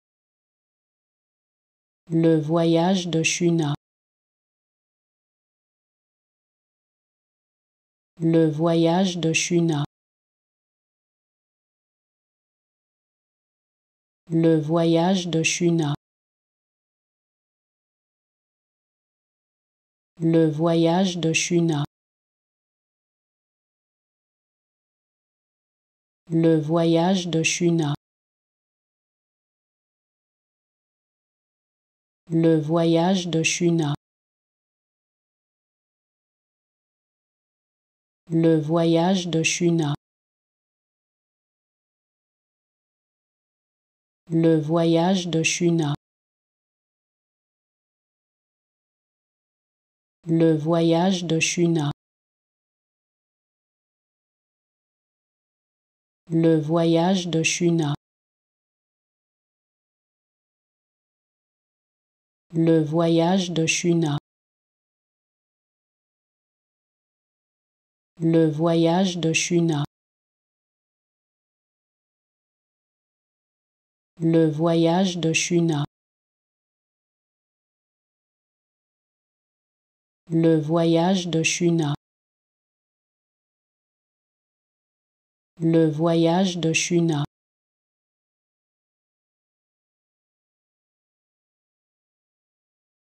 How to Pronounce ''Le Voyage de Shuna'' (Shuna's Journey) Correctly in French
Learn how to say and properly pronounce ''Le Voyage de Shuna'' in French with this free pronunciation tutorial.